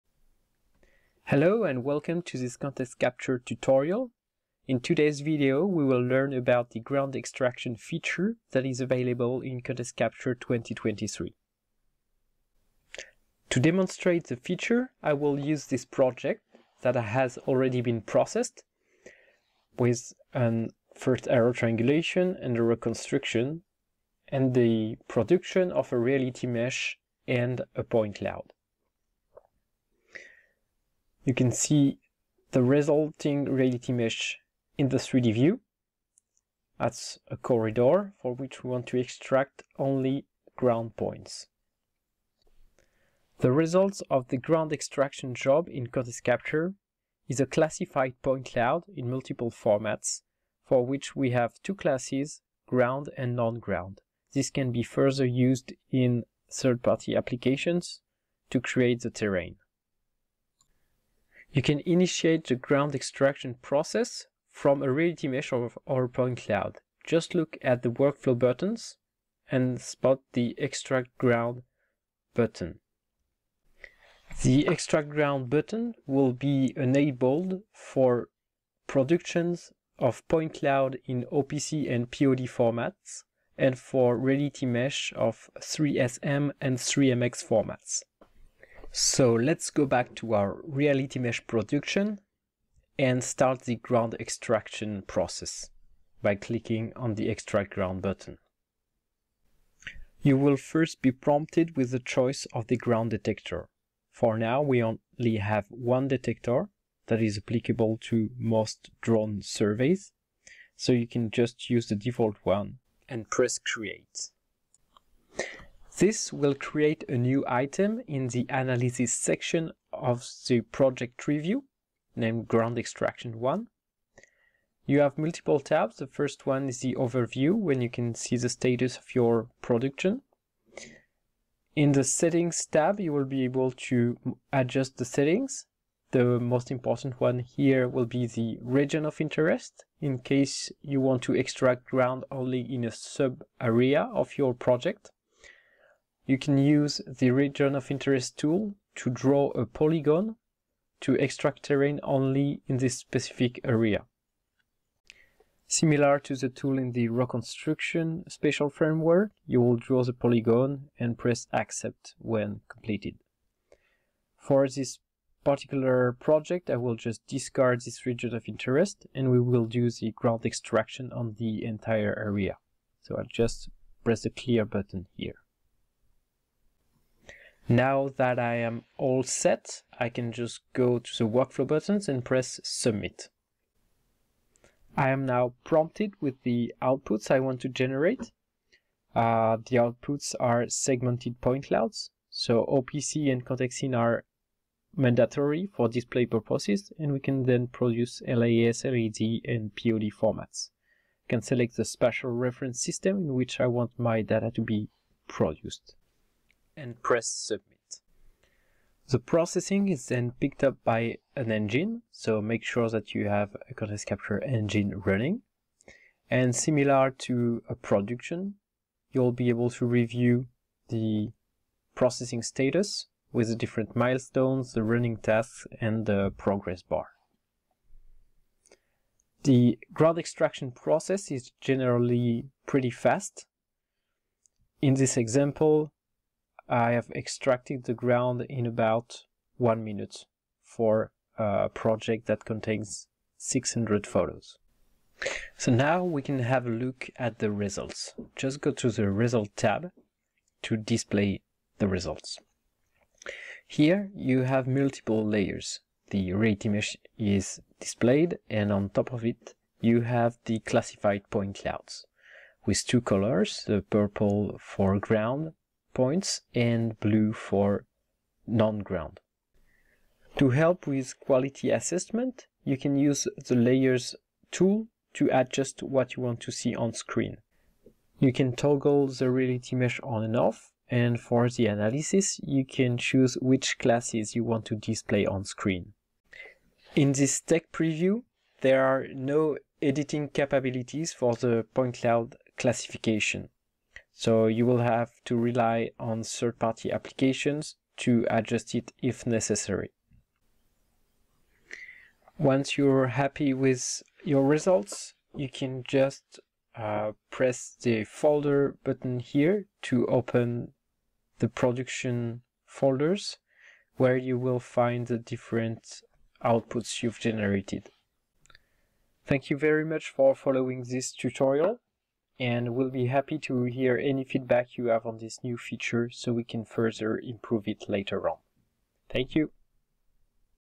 iTwin Capture 2023 - Tutorial: Ground Extraction
In this video, you will learn how to trigger a ground extraction job to generate a classified point cloud with two classes (ground and non-ground).
This feature is in Tech Preview and we are looking forward hearing from you to help improve it.